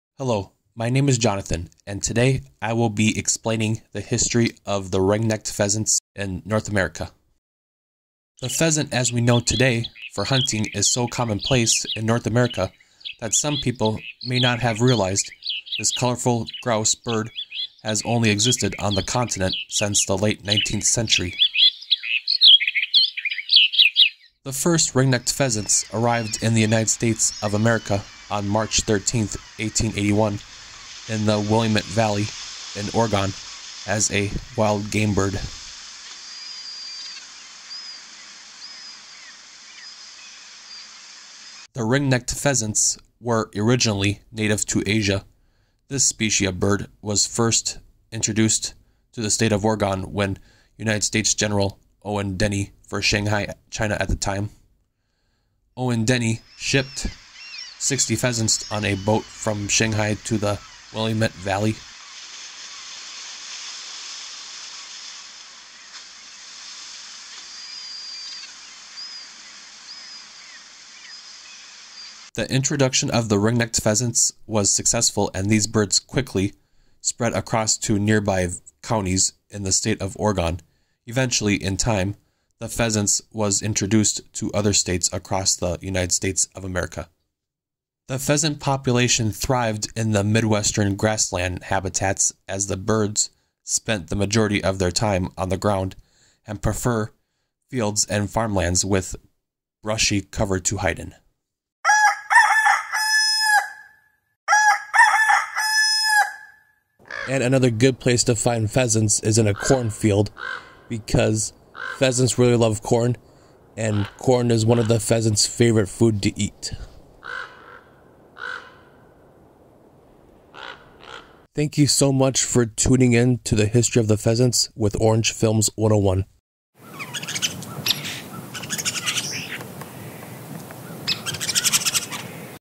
History Of Ring-Necked Pheasants
Jonathan the animated avatar explains the history and origins of the ring-necked pheasants. And how this type of game bird got to North America. And became one of the invasive species.